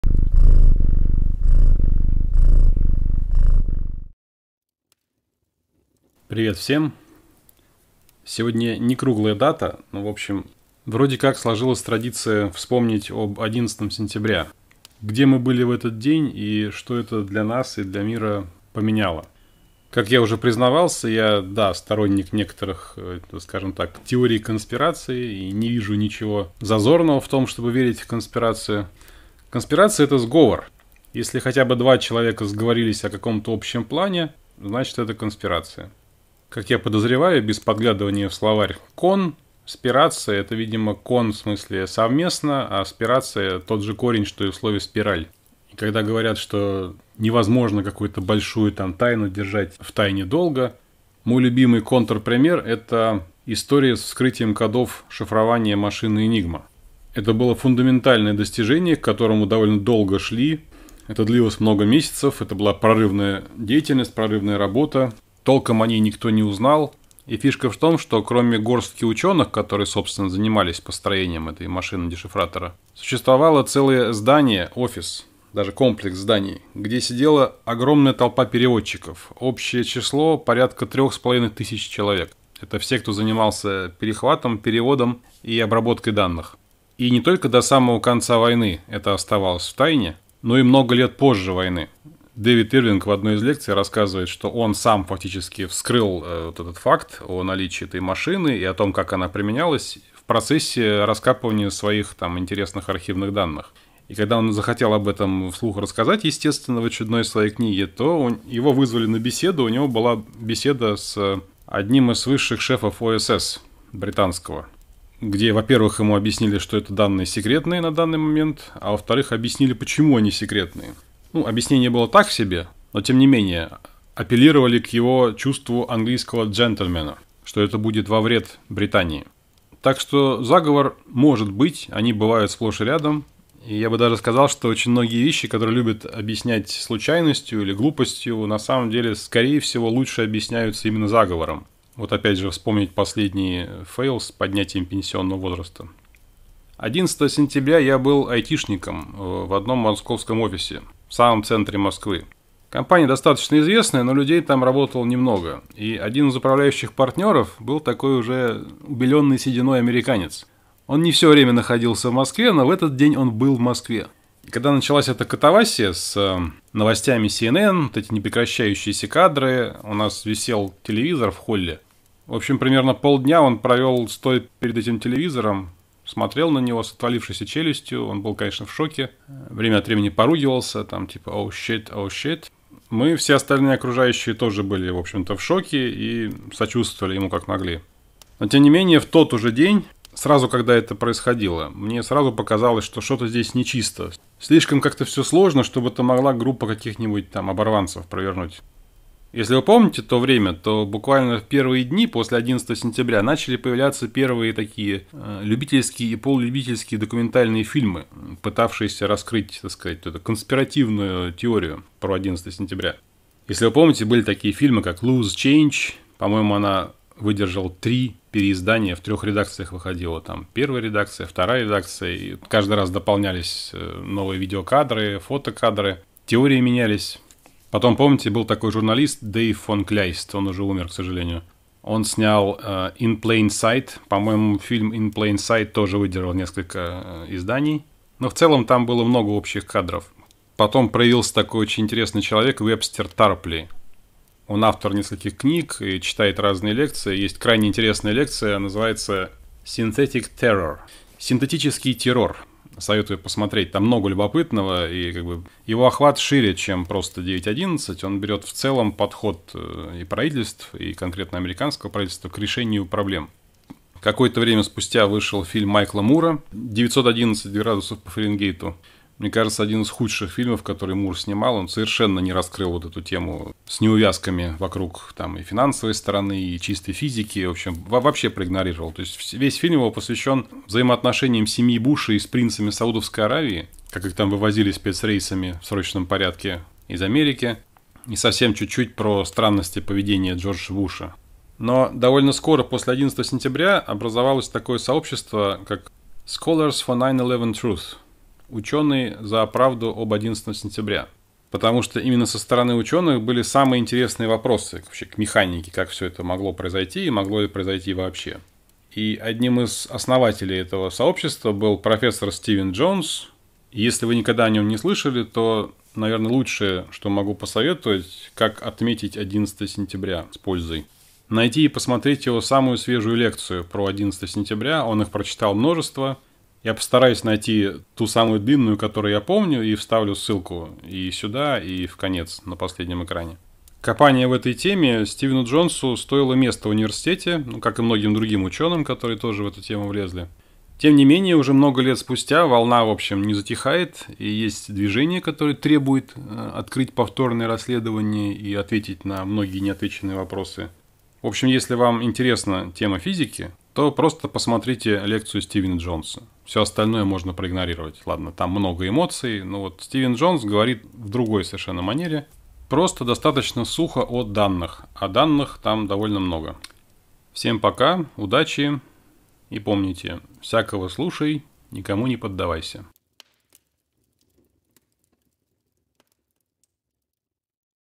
Занимательная физика 9/11
Как помянуть 11 сентября?
Можно провести факультатив по физике.